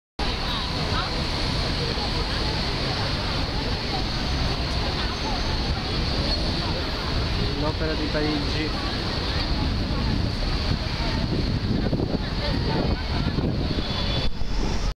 Paris - Opéra de Paris [MiniClip] September, 2012
Home movies made ​​with pocket camera SAMSUNG HMX-W200TP on September, 2012.

by G&C Travels